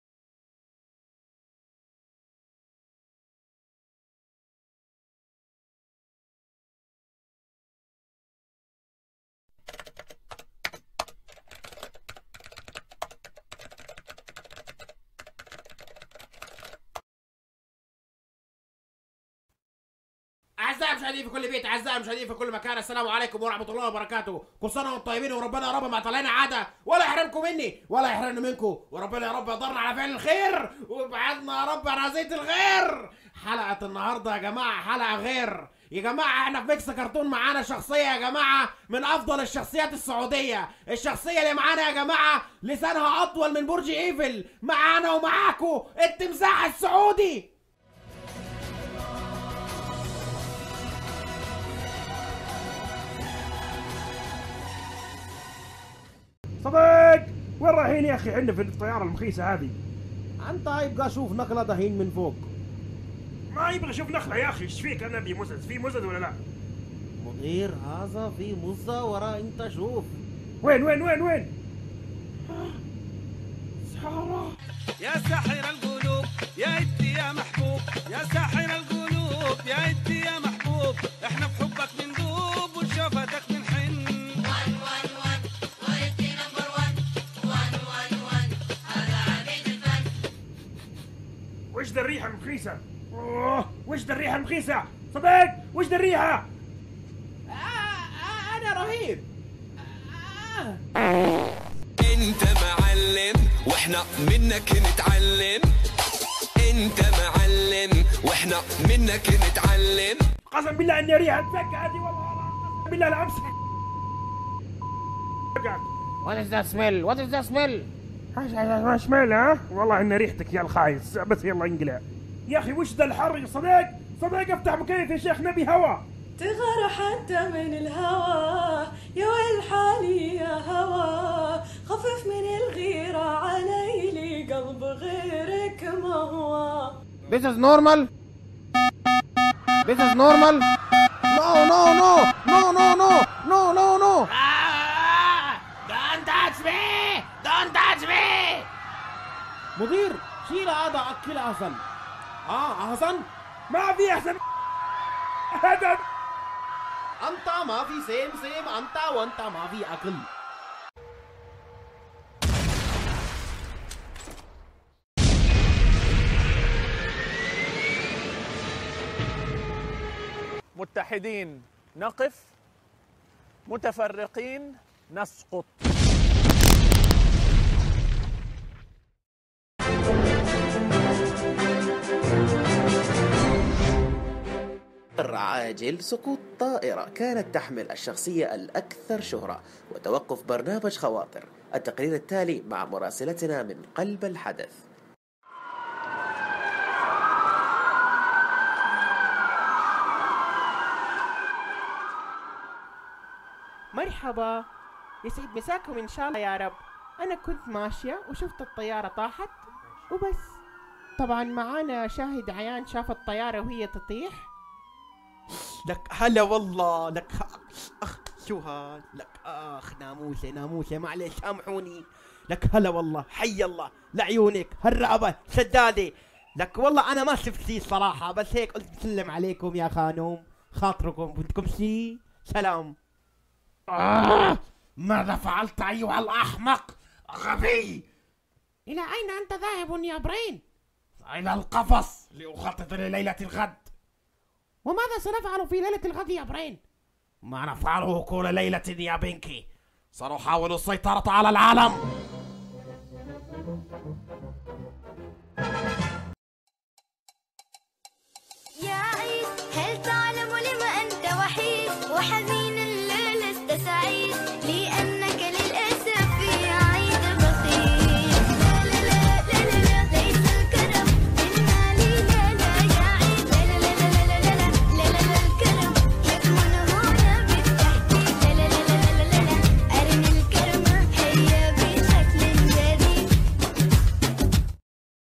مكس كرتون mix cartoon الحلقة 2 " رامز جلال يعمل مقلب بالتمساح "
مكس  كرتون هو عبارة عن خليط بين المشاهيرالواقع و الكرتون القديم والحديث
الحلقة الثانية : رامز جلال , التمساح , سارة اليافعي , فيصل الخالد, بلقيس , سلتوح , أحمد الشقيري , فيصل الزهراني , نصرة الحربي , نمس , بنكي وبرين

رسم وتحريك :
احمد ابوشوارب

تقليد جميع الاصوات المبدع :
ناظم ظفر
Instgram & Snapchat /nazemzafar